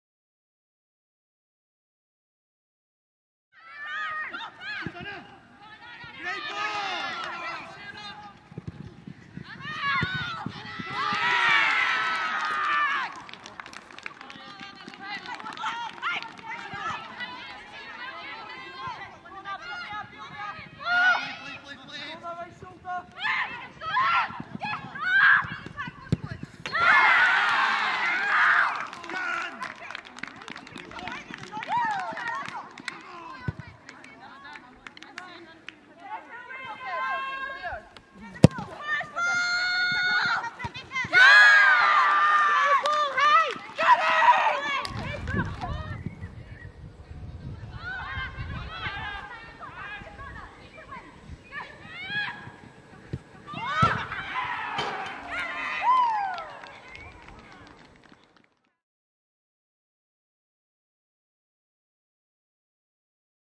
🎥 UNIQUE ANGLE: Celts to the four against Spartans at Lennoxtown
Fran Alonso’s side raced to a convincing 4-0 victory over Spartans in Lennoxtown, as Sarah Ewens, Jacynta, Jodie Bartle and Mariah Lee secured all three points for the Hoops.